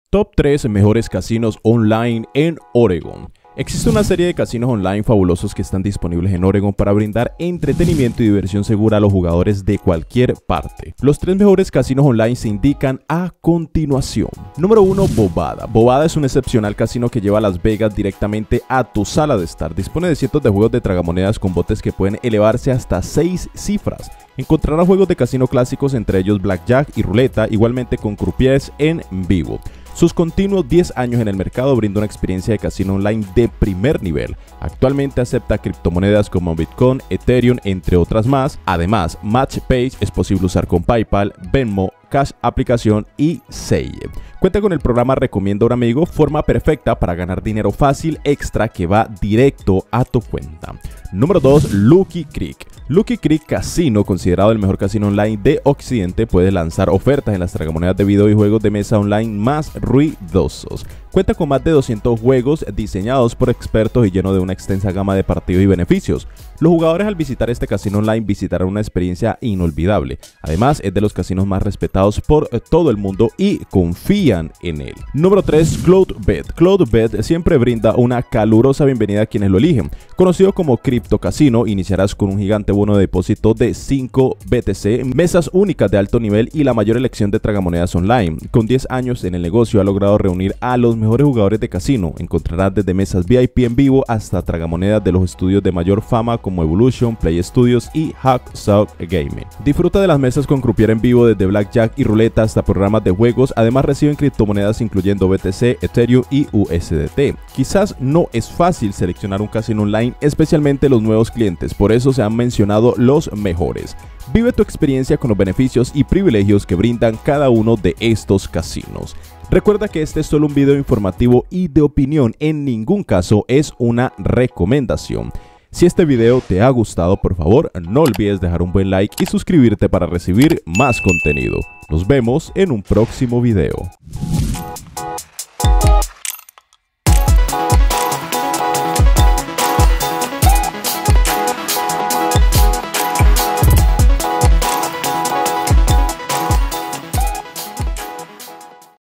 🥇 3 mejores CASINOS ONLINE en OREGÓN en 2025
¿Cuáles son los mejores casinos de Oregón (USA) online? En este vídeo te lo explicamos.

NOTA LEGAL